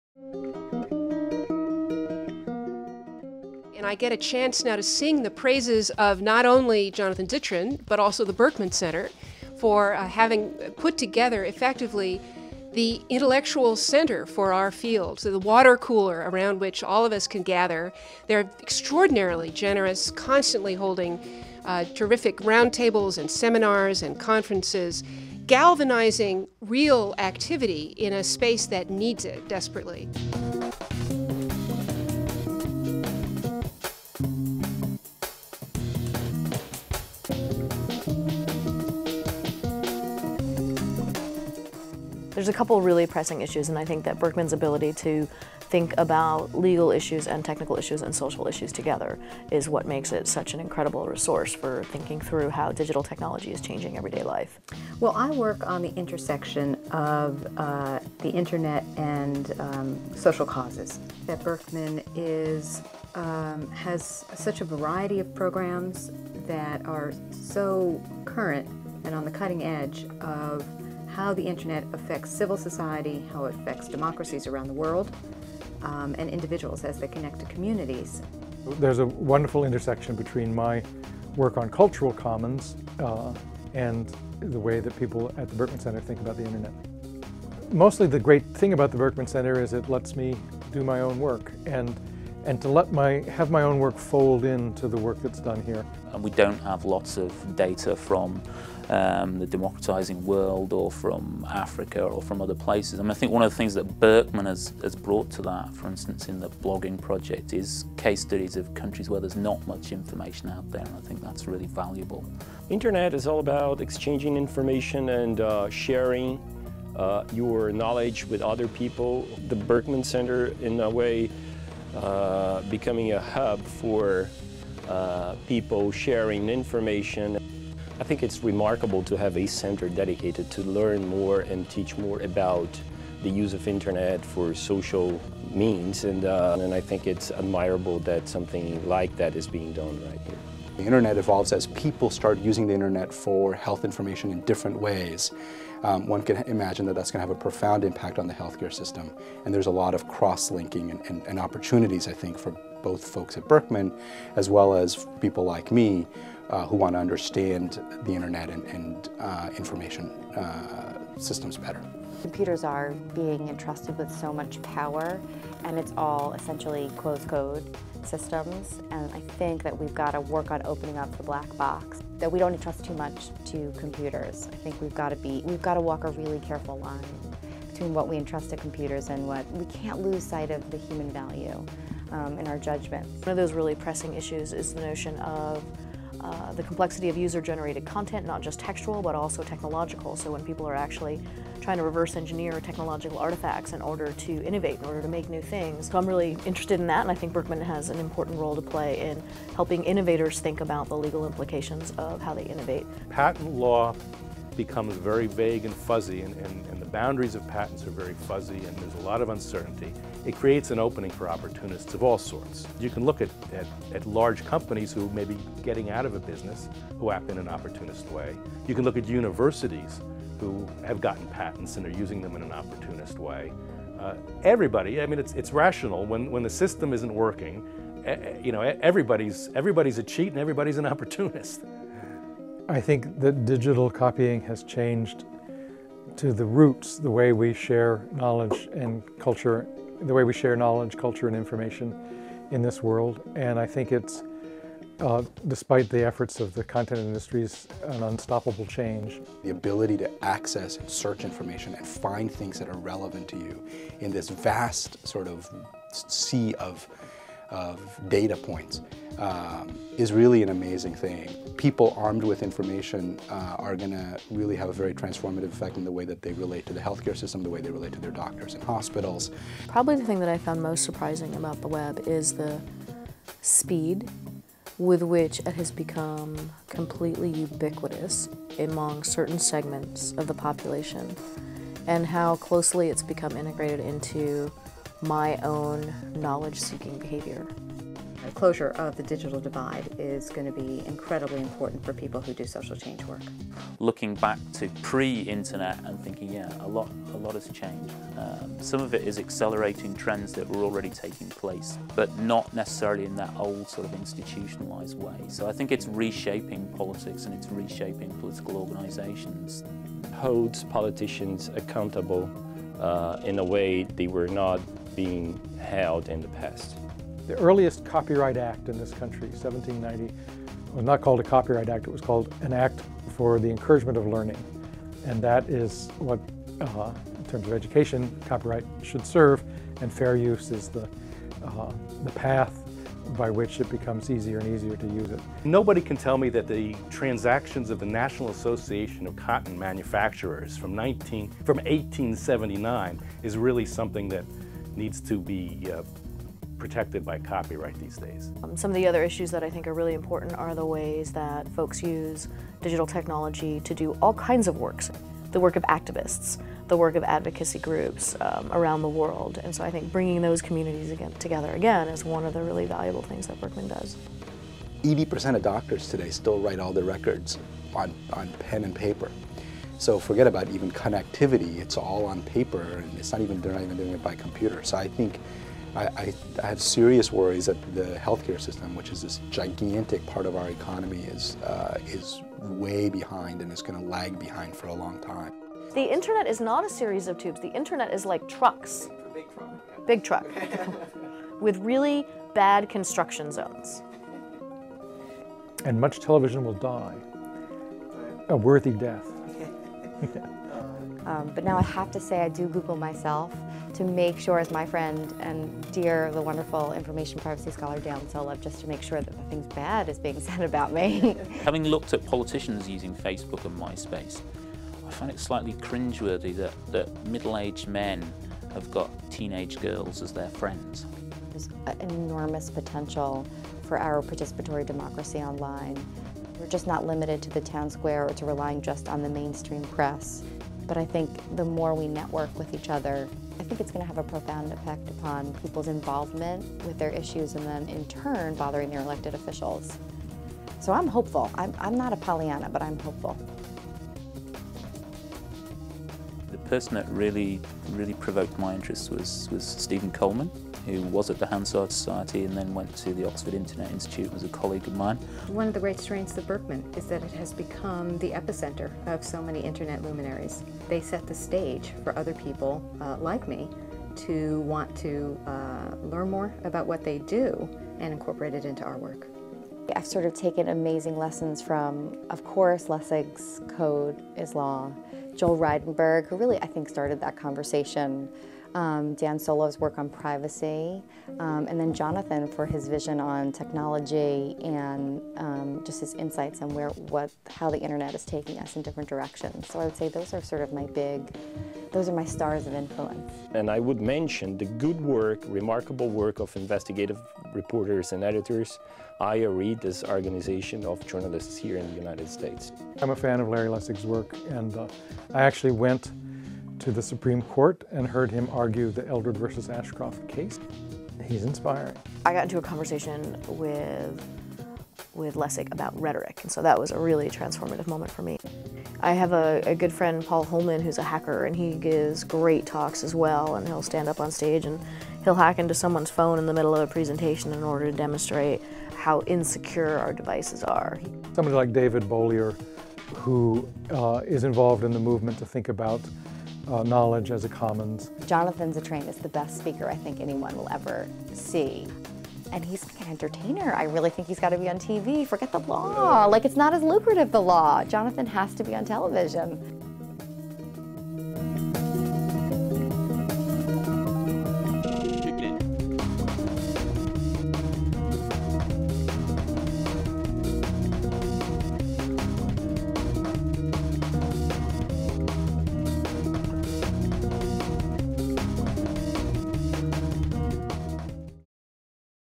Berkman ONEonONE: Reflections
Recent luncheon guests reflect on technology, law, and the way digital culture is altering the fabric of contemporary life.